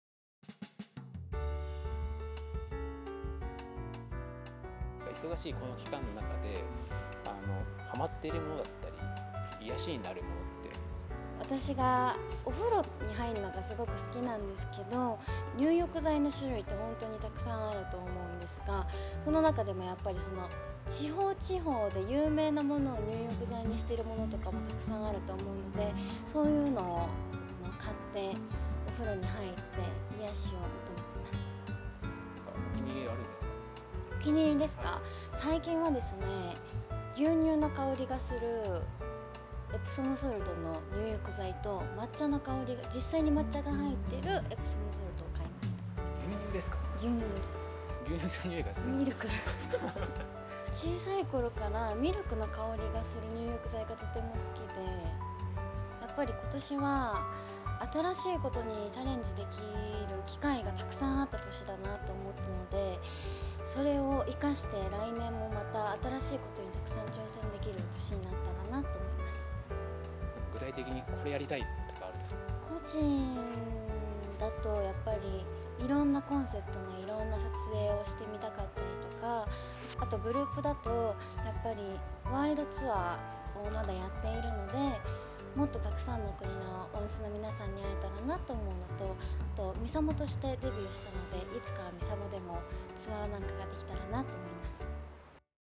TWICE・SANA「お風呂に入るのがすごく好き」 多忙な日々を癒やすアイテム明かす
TWICEのSANAさん（27）にインタビュー。自身の癒やしや、2024年の抱負を教えてくれました。

TWICEとしてワールドツアーを行うなど活躍しているSANAさん。化粧品ブランド『イヴ・サンローラン・ボーテ』のジャパンアンバサダー就任会見後にインタビューに応じてくれました。